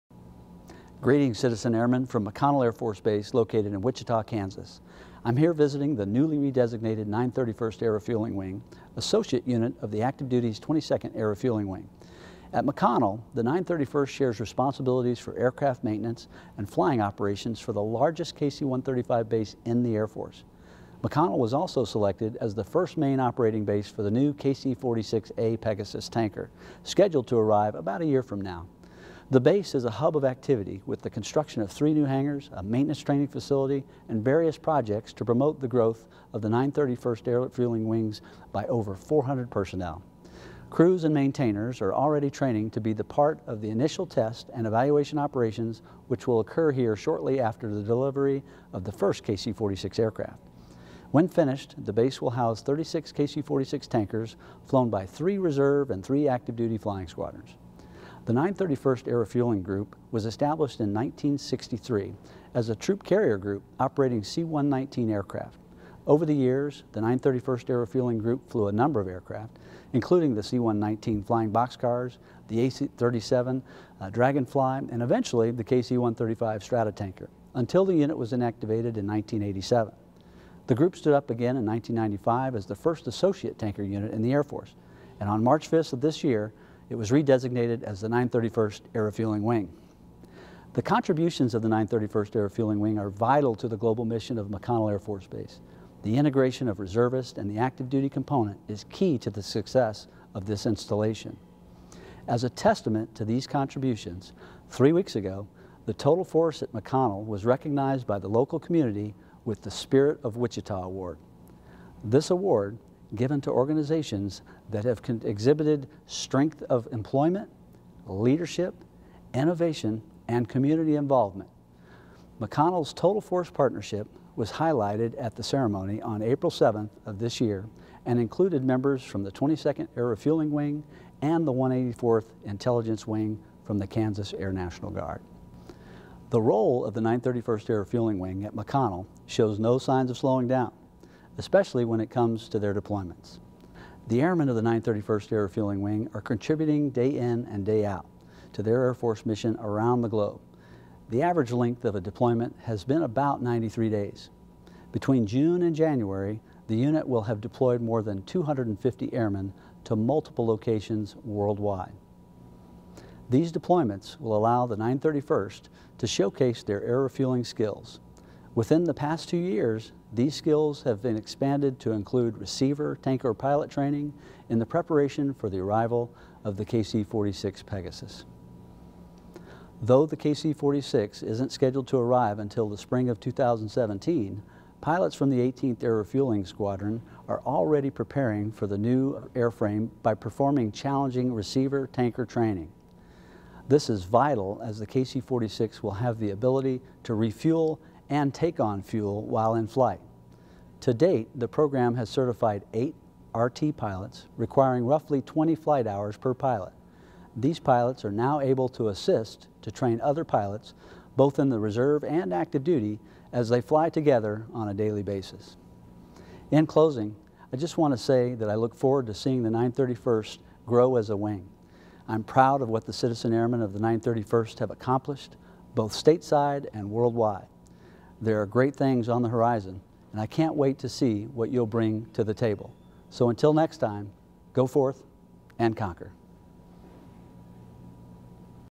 General Flournoy Speech
Fourth Air Force Commander, Maj. Gen. John C. Flournoy, recently visited Team McConnell for the 931st Air Refueling Wing Re-designation Ceremony.  While here, Flournoy took time to review the unit's mission and express his excitement at the future and growth of the wing.  (U.S. Air Force video by Airman 1st Class Joshua Crawley)